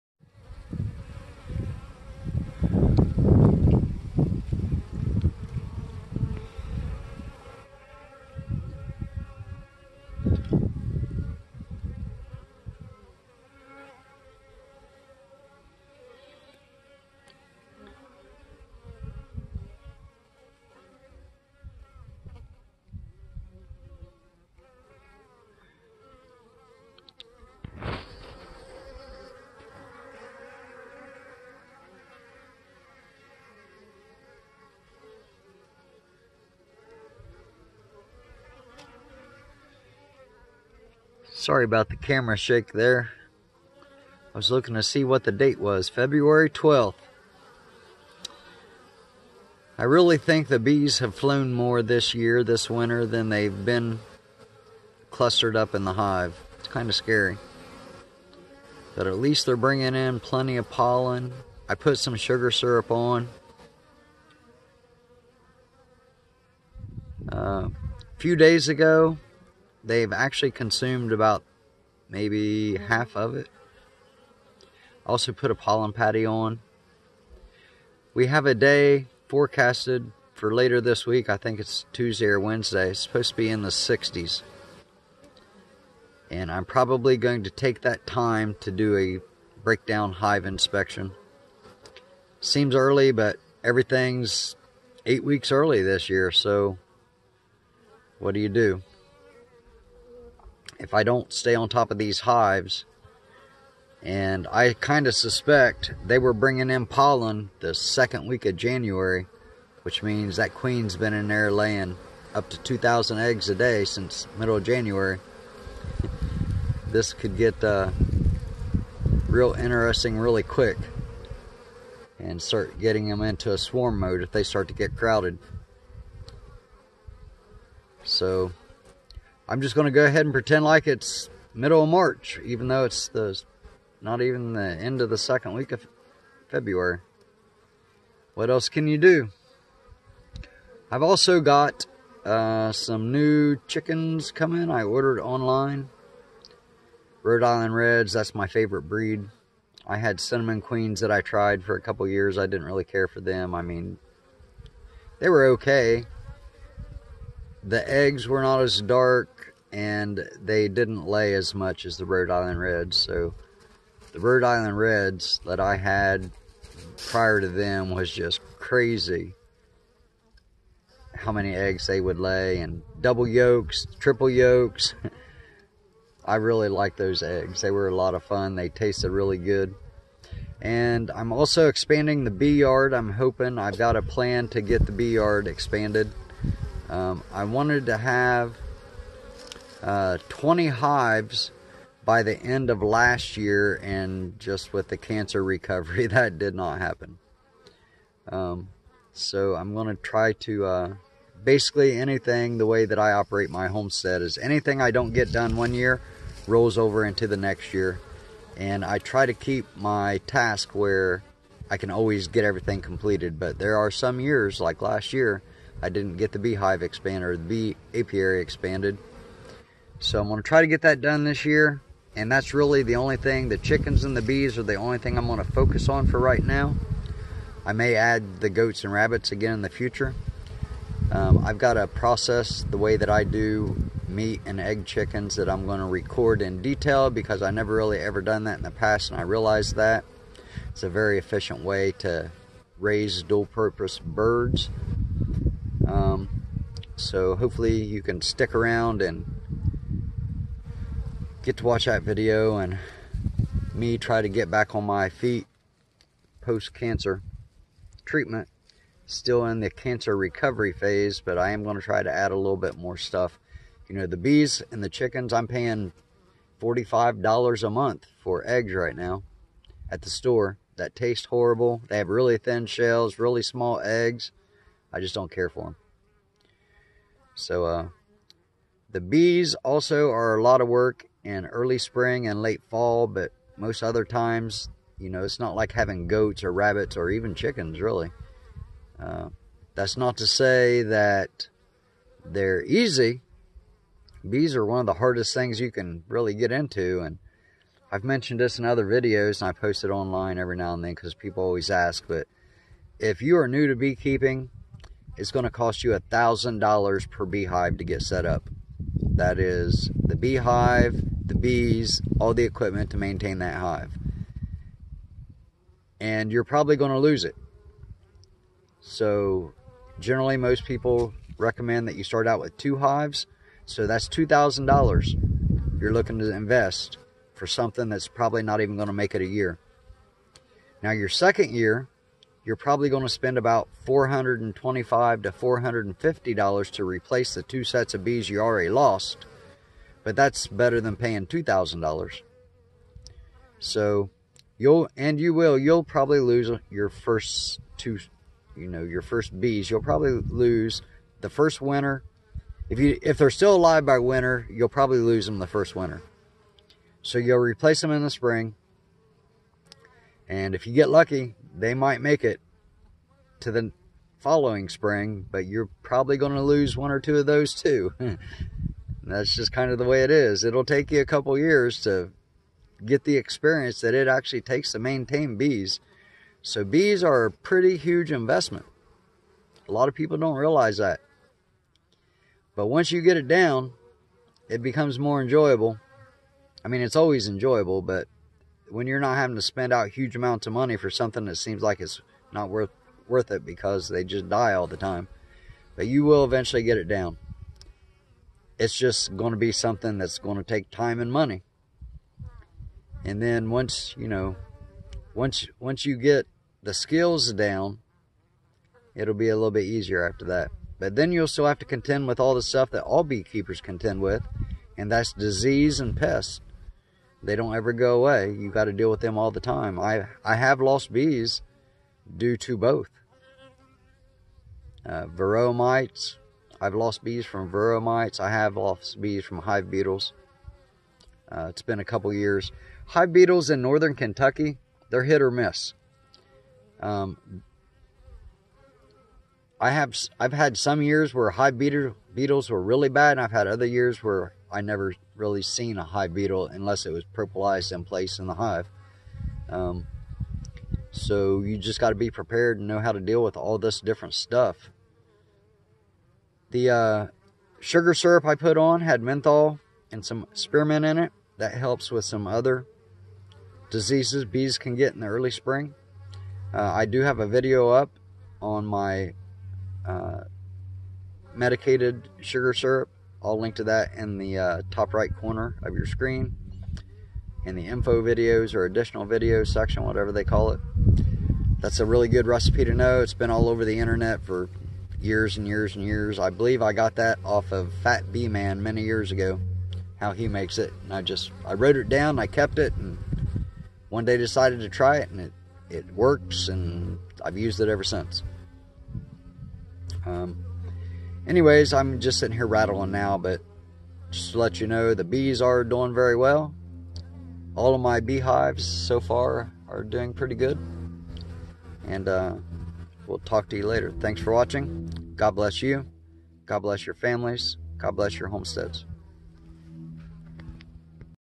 feb 12 2023 northern kentucky bee hive watch
Its only February 12, 2023 in northern kentucky and the bees have flown more this winter than they have been clustered. Highs are going to hit mid-60s this week. I'm going to force myself to pretend its mid April and break the propolis seals for bee hive inspections because i know they have been bringing in pollen since mid-january. I hope i dont make a mistake. Also I talk about some upcoming plans on the homestead.